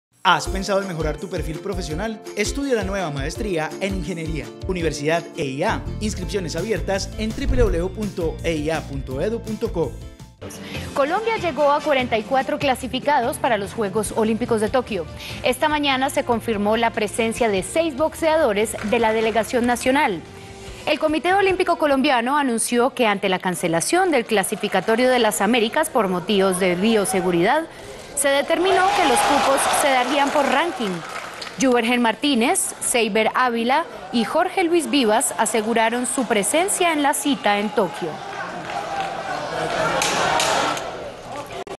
Boxeadores antioqueños clasificaron a Juegos Olímpicos de Tokio
Colombia llegó a 44 clasificados para los Juegos Olímpicos de Tokio. Se confirmó la presencia de seis boxeadores de la delegación nacional.